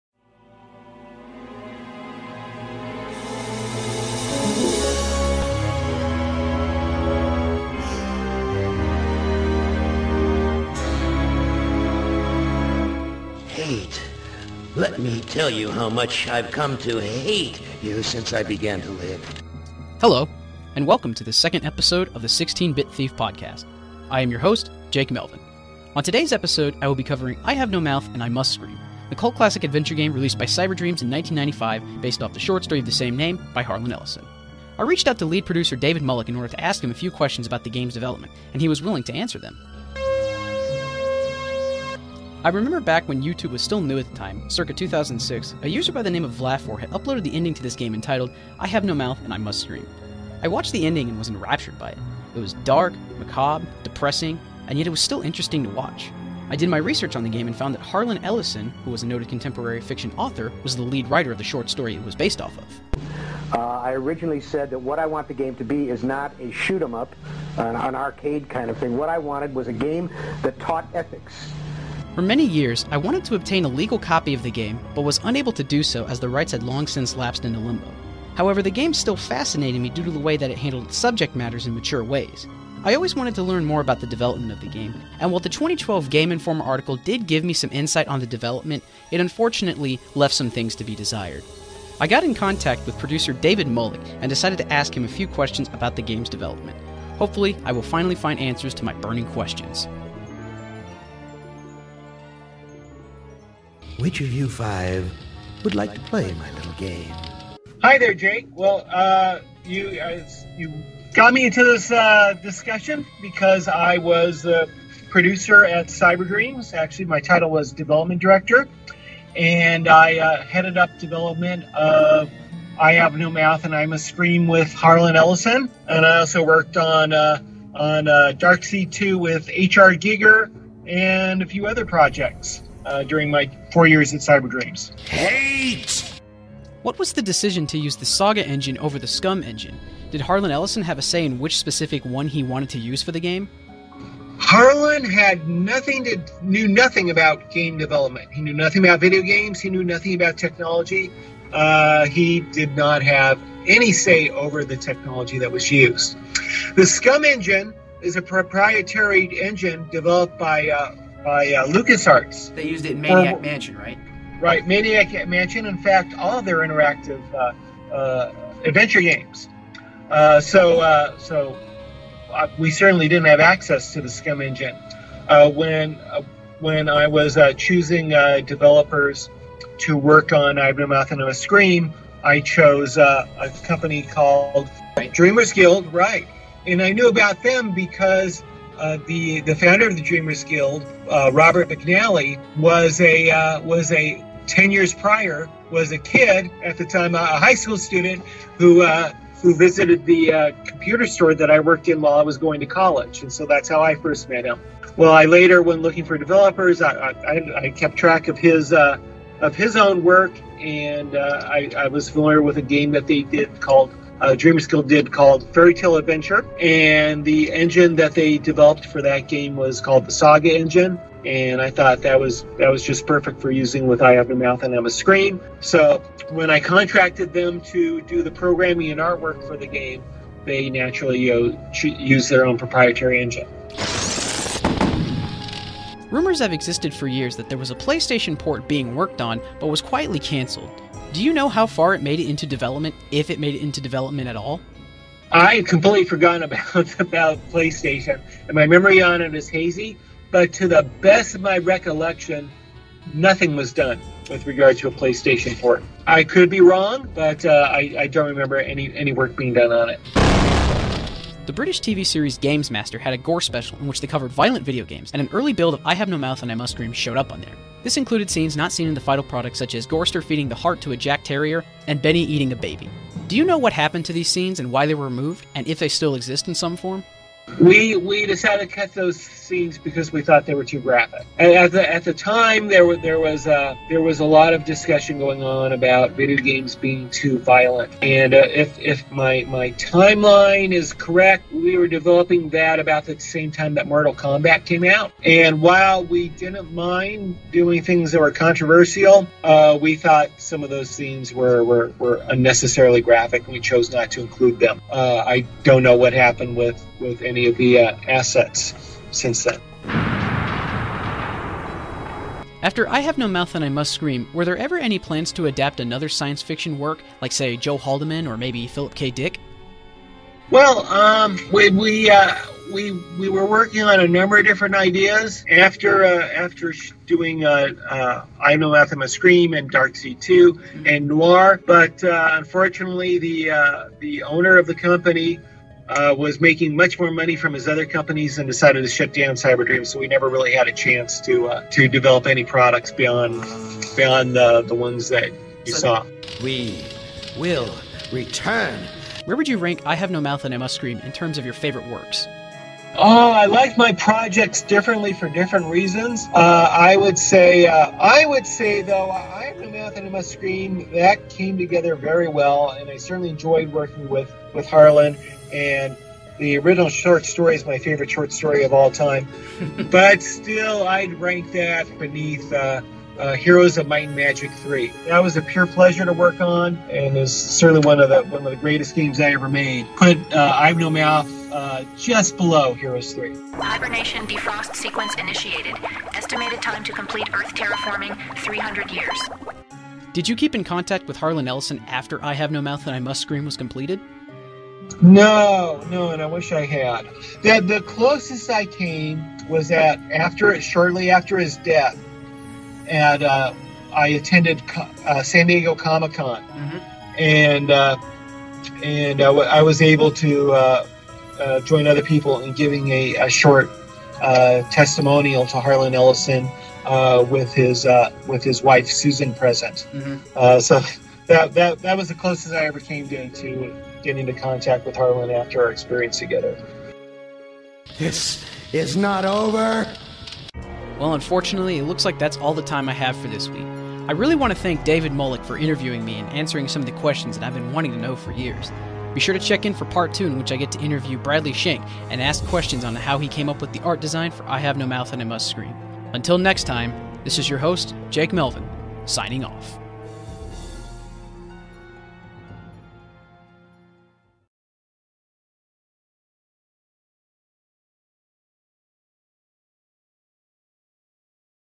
16-Bit Thief Episode 1 - David Mullich
The first true episode of my podcast, featuring legendary game developer David Mullich of Cyberdreams, 3DO Company, and Disney Computer Software fame. Done as a final for my podcasting class, this episode was made to hit all the bells and whistles required for the final grade, but I put my own spin on it, as I wanted to inquire more about I Have No Mouth and Cyberdreams.